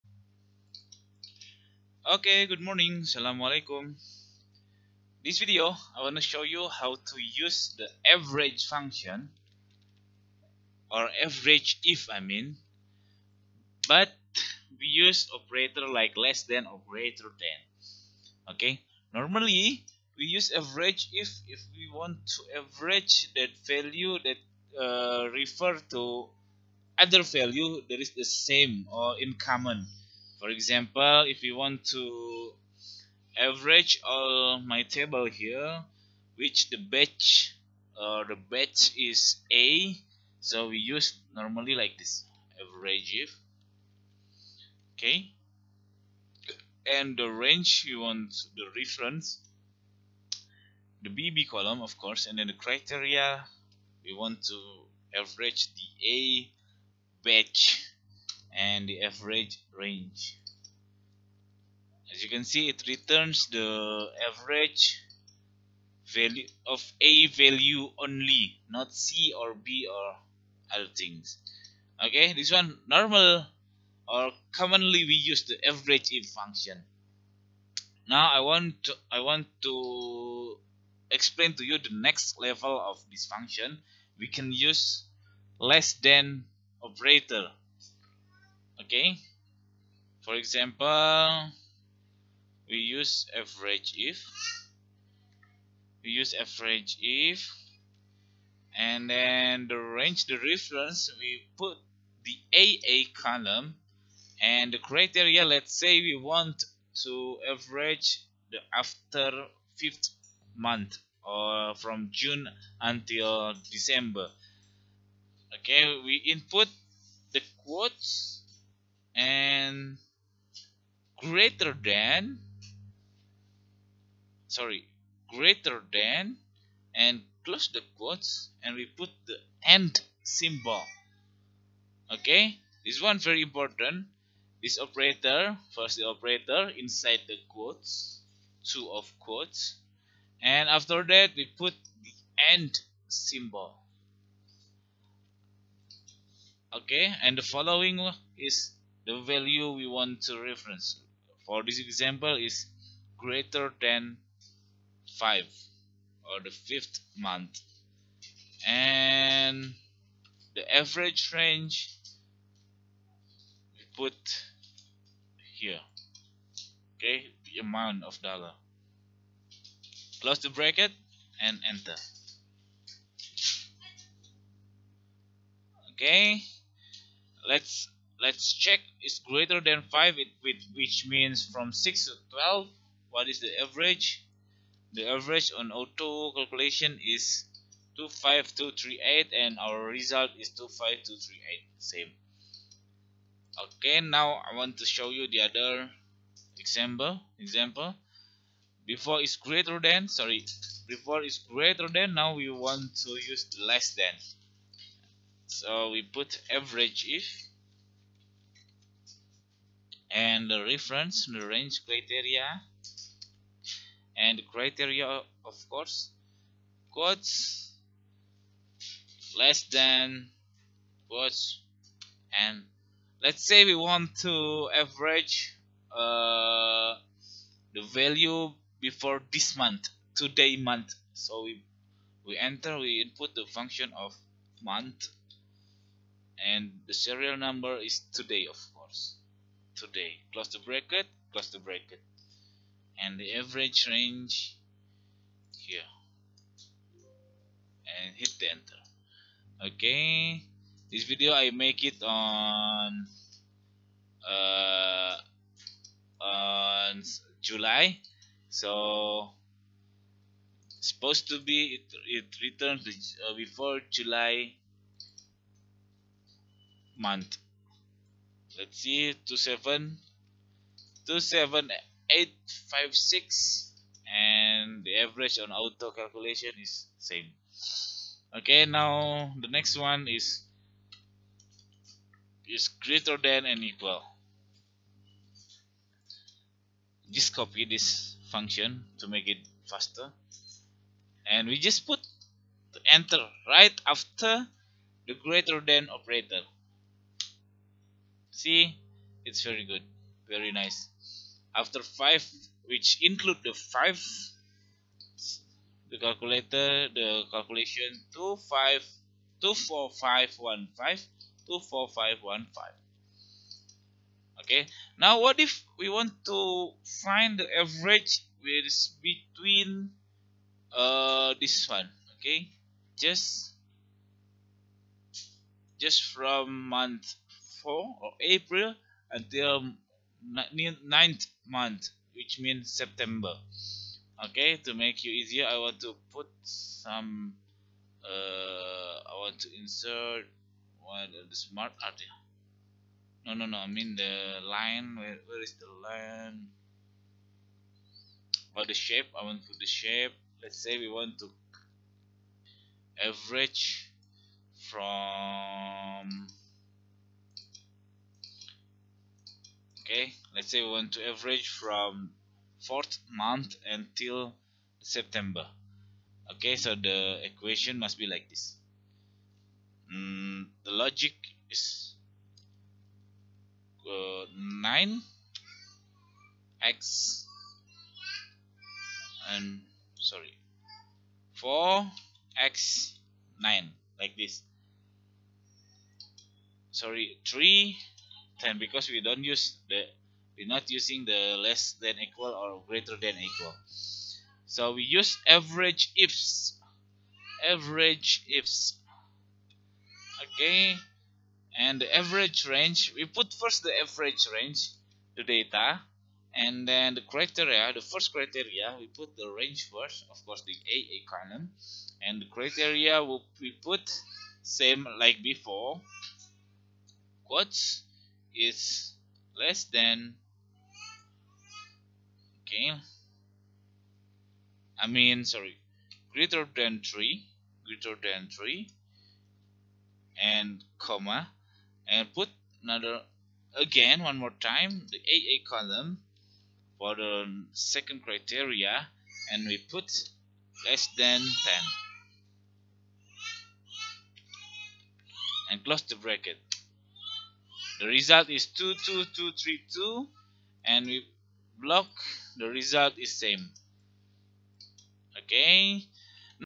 Excel to use greater than or less than on AVERAGEIF function
Averaging with specified condition is could be done by using AVERAGEIF function, commonly we use this function just to refer the range to static value, but actually AVERAGEIF can do averaging to value with condition less than or greater than. You can watch & learn from this video.